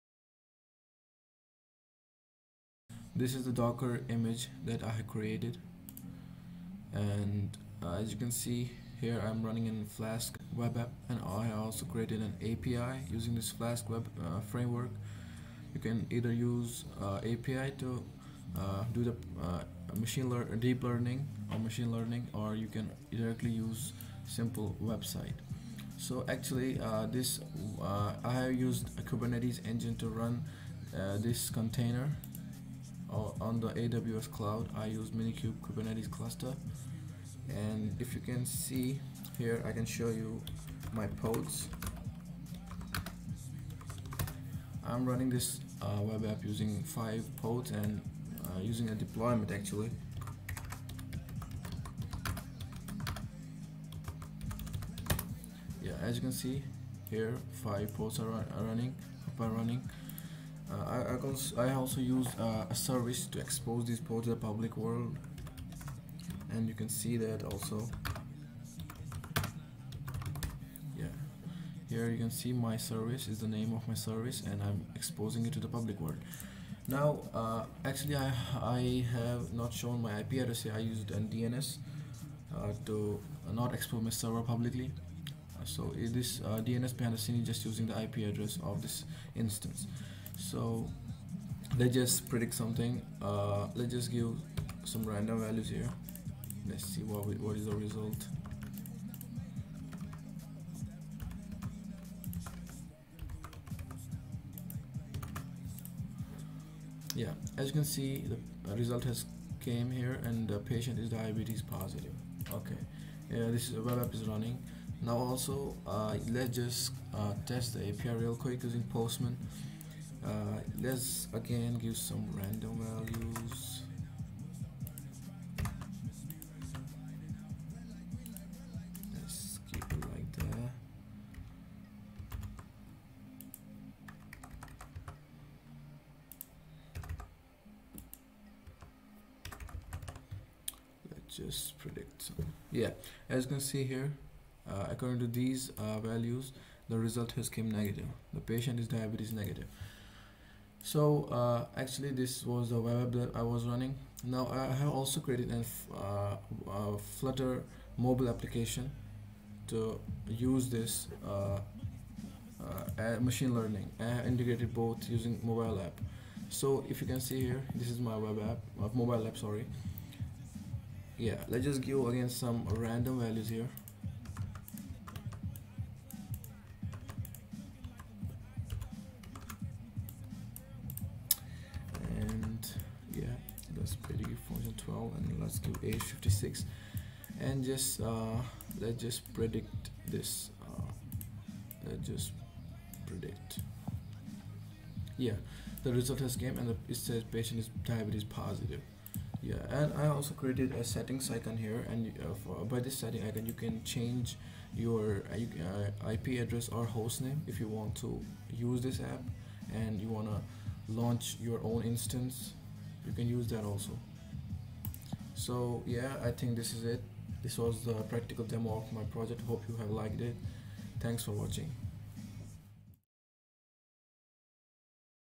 Diabetes Prediction using Deep Learning with integration of Web App, Mobile App, and API. 🔥🔥
This project integrates Deep Learning with Web App, Mobile App, and API. 🔥🔥

In this project, I have used "Pima Indians Diabetes Dataset" to create a deep learning model with almost 80% accuracy. Then I created a Dockerfile to create a container image in which I am running a flask app and used this model in it. For deployment, I am using AWS Cloud and I have launched minkube on an EC2 Instance for running my Kubernetes Cluster. This K8s cluster is running a deployment containing 5 pods of my container image. Finally, I used a service to expose my deployment to the outside world.